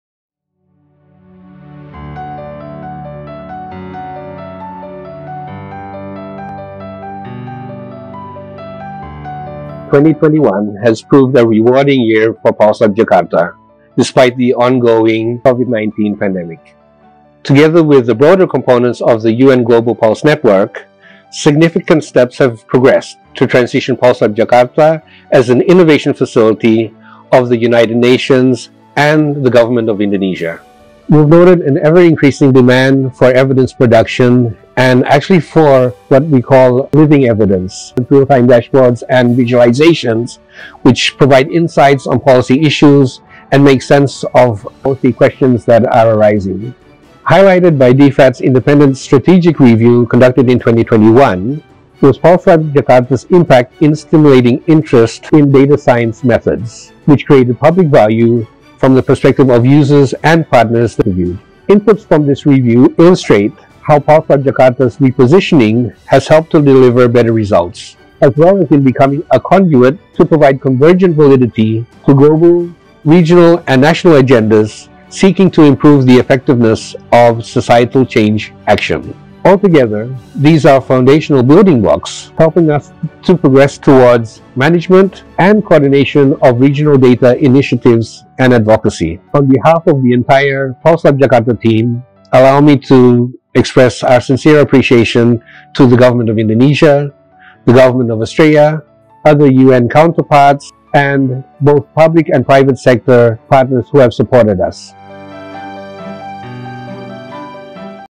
Head of Pulse Lab Jakarta: 2021 Annual Report
Petrarca Karetji (Head of Pulse Lab Jakarta) speaking on the Lab's 2021 Annual Report